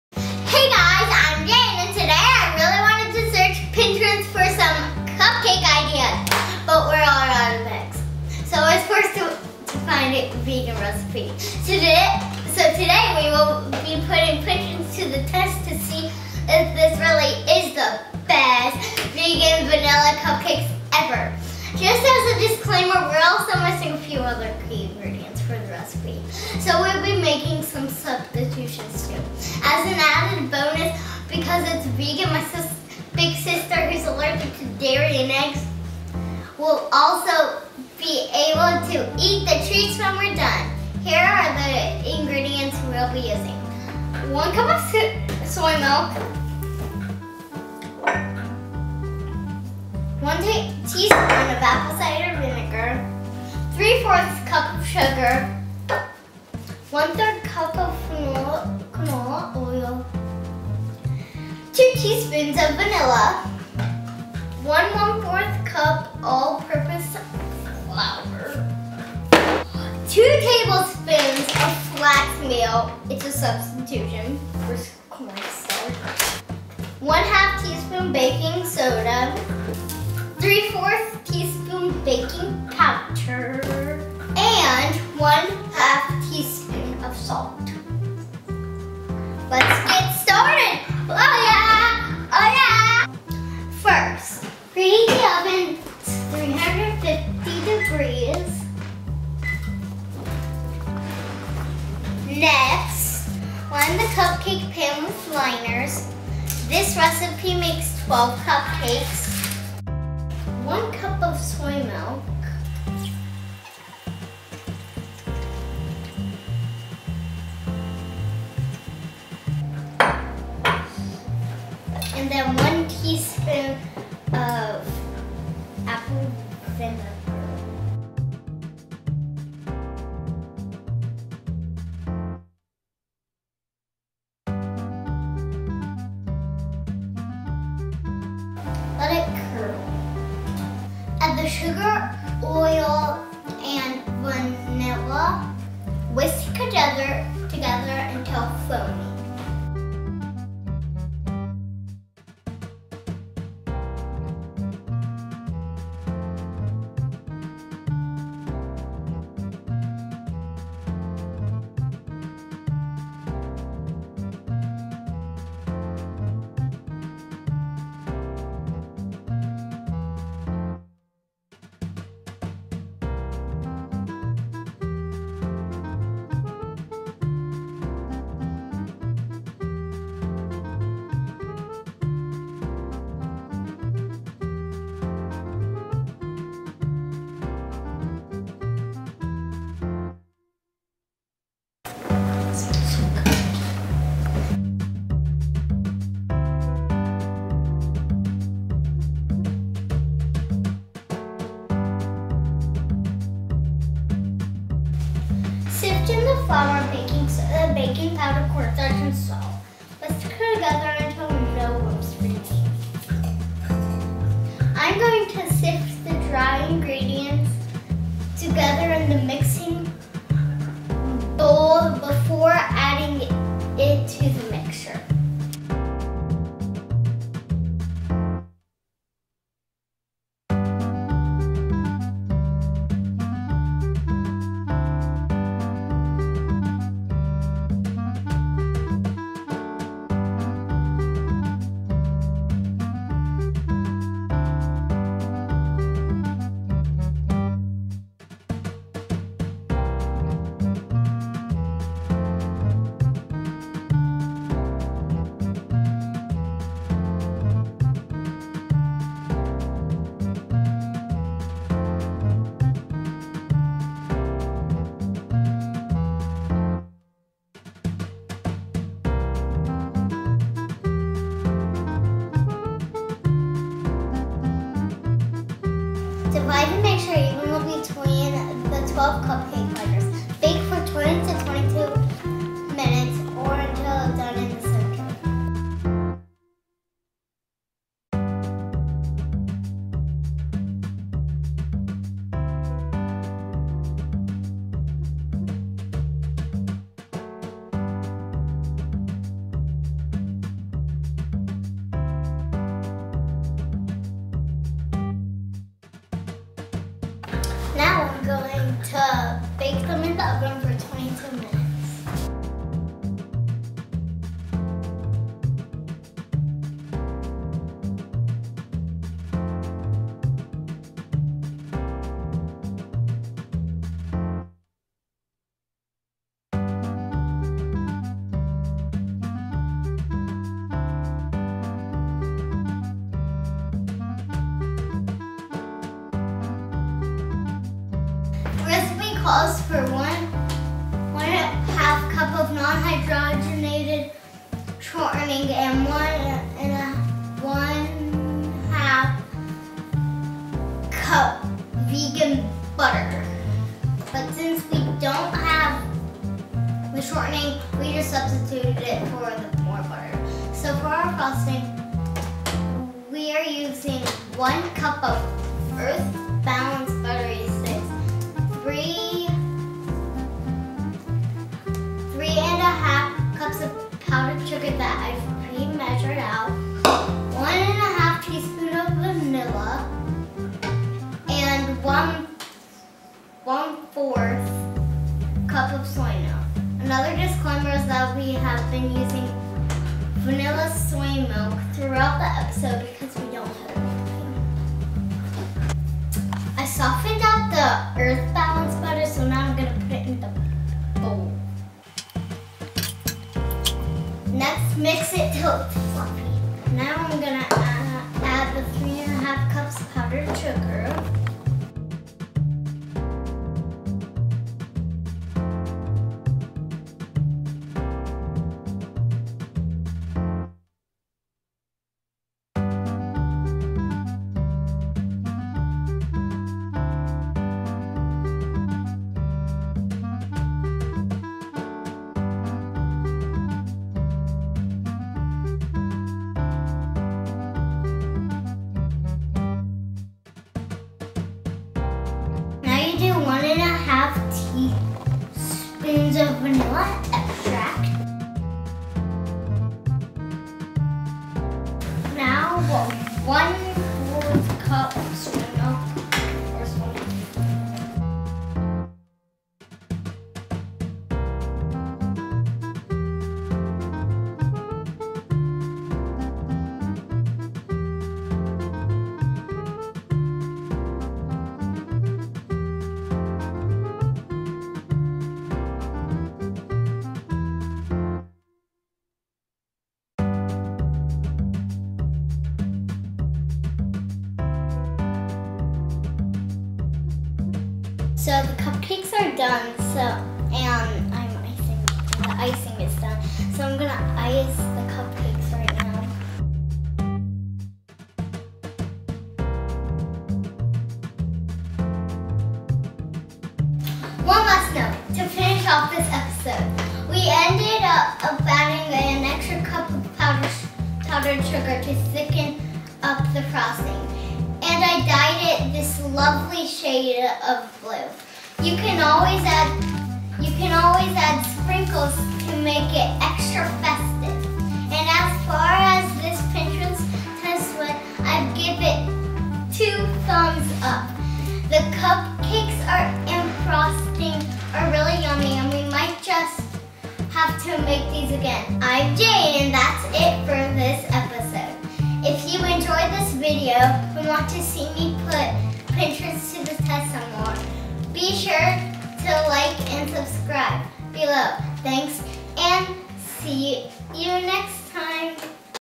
How to Make Vegan Vanilla Cupcakes
In this video, Jane finds a vegan vanilla cupcake recipe on Pinterest and puts it to the test.  Will it measure up?  Watch and find out!